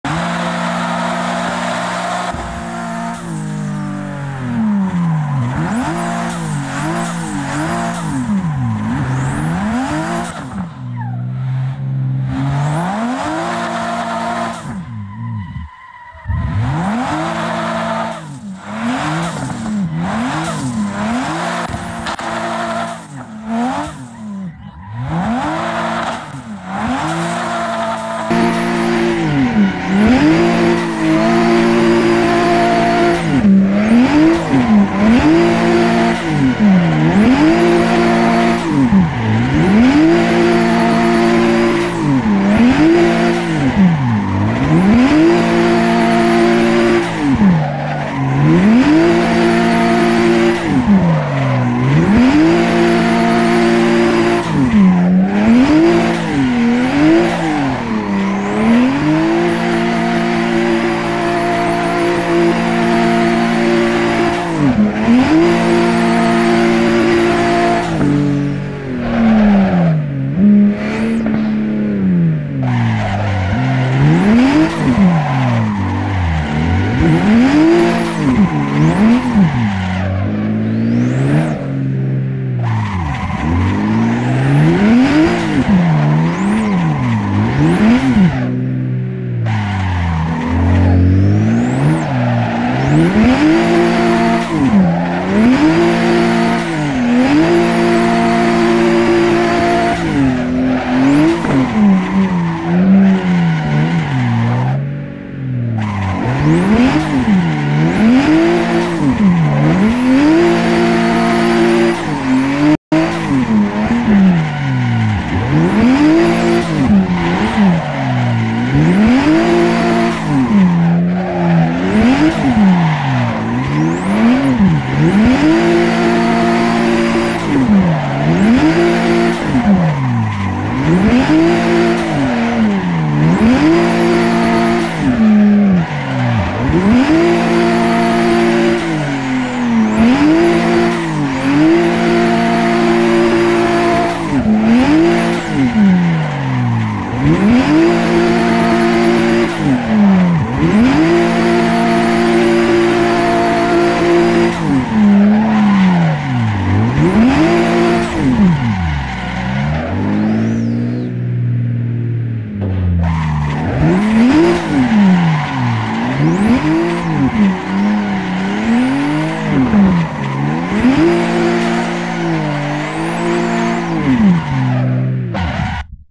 Mixed Reality Drift
combining vr capture from my quest 3 with replay capture for a different drift experience on my moza r3 setup in my big rig.